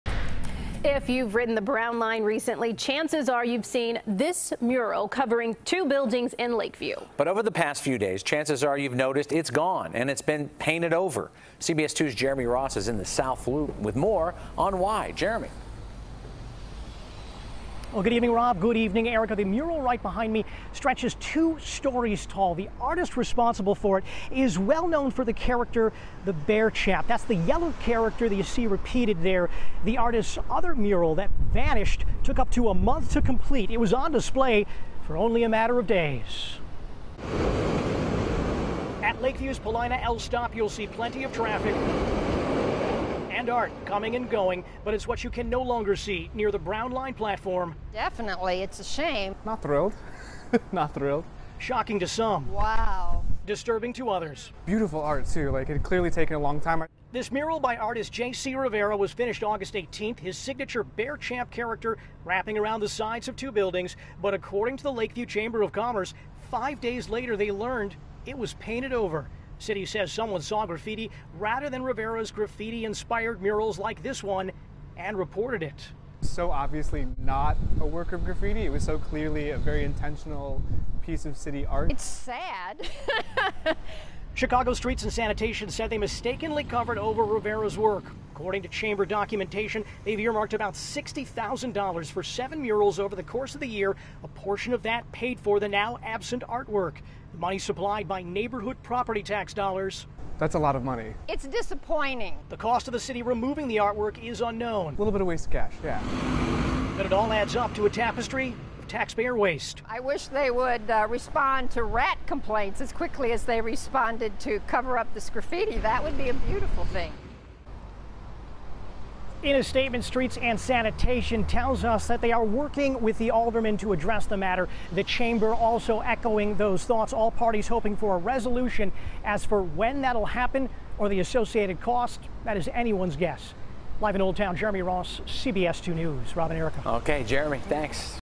Commissioned Mural Painted Over In Lakeview, Mistaken As Graffiti
A large mural, commissioned by the Lakeview Chamber of Commerce with property tax dollars, was destroyed just days after the it was completed after it was mistaken as graffiti.